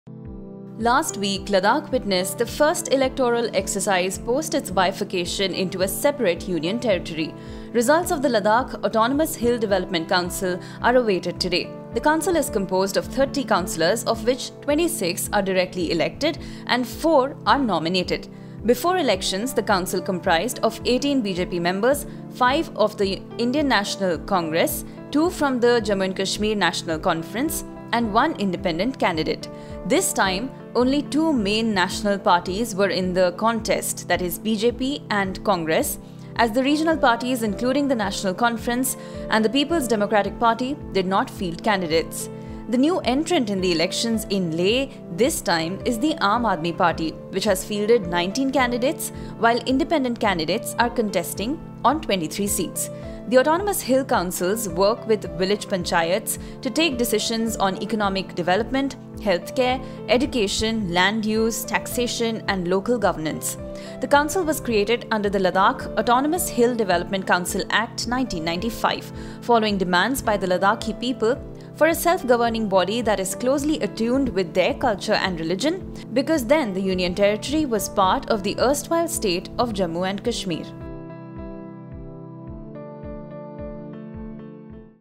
Ladakh to get new Autonomous Hill Development Council | Oneindia News
Last week Ladakh witnessed the first electoral exercise post its bifurcation into a separate Union Territory on August 5th, 2019. Results of the Ladakh Autonomous Hill Development Council are awaited today. The council is composed of 30 councillors of which 26 are directly elected and 4 are nominated and these members take decisions on local issues.